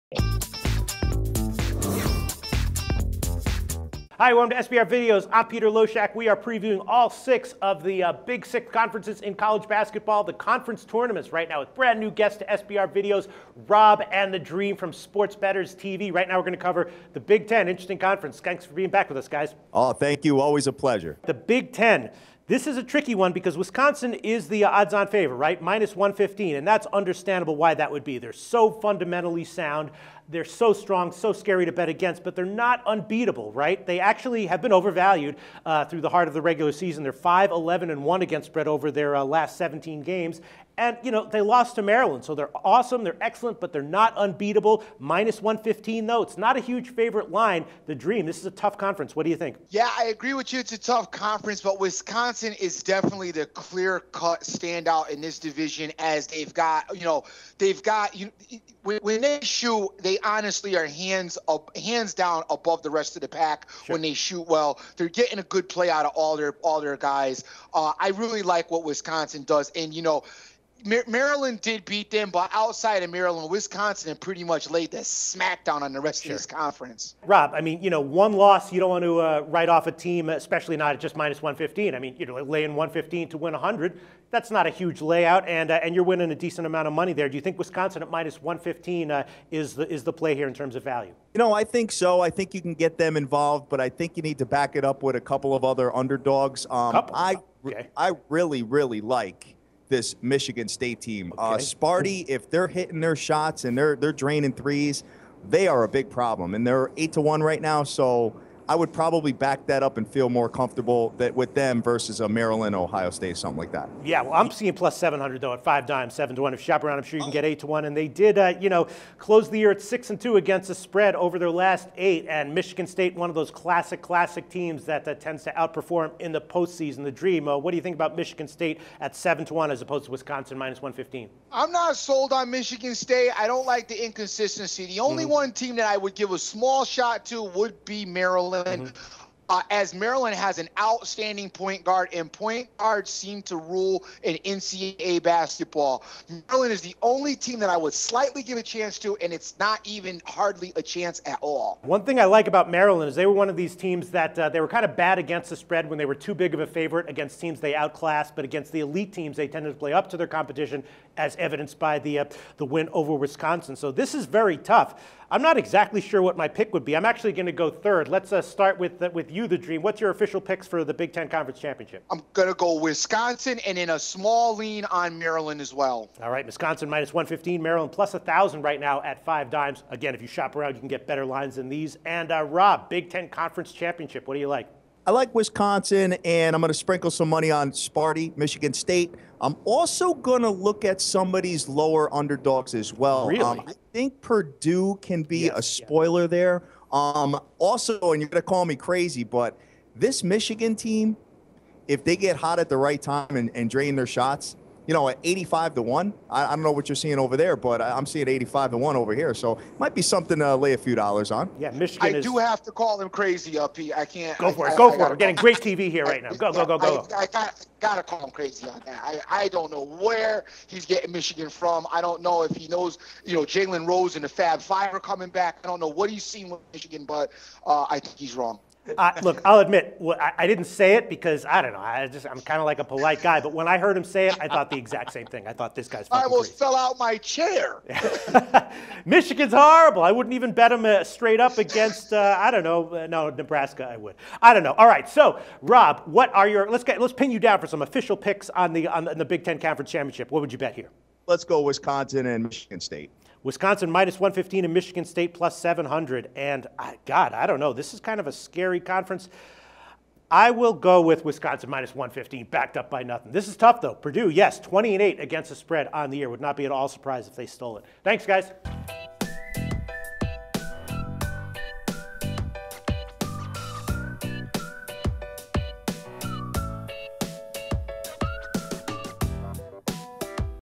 NCAA Basketball Picks: Big 10 Tournament & Futures Predictions
Don't miss out on all the NCAA basketball betting action that could bring you top profits in the Big Ten tournament happening this week! Our college basketball odds pros take a look at the tournament and discuss futures for your NCAA basketball picks with the Big Ten conference here on Sportsbook Review.